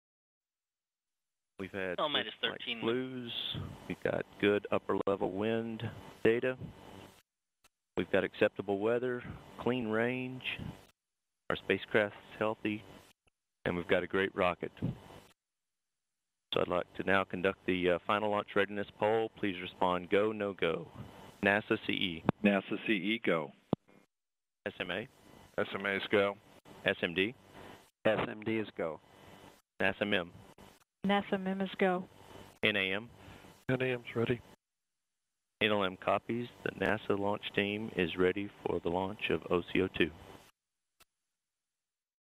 OCO-2 & Delta II Ready for Launch
In the Launch Control Center at Vandenberg Air Force Base, NASA and contractor managers and engineers monitor progress in the countdown to launch a Delta II rocket. The launch vehicle will boost the Orbiting Carbon Observatory, or OCO-2, satellite on a on a mission to study the Earth's atmosphere.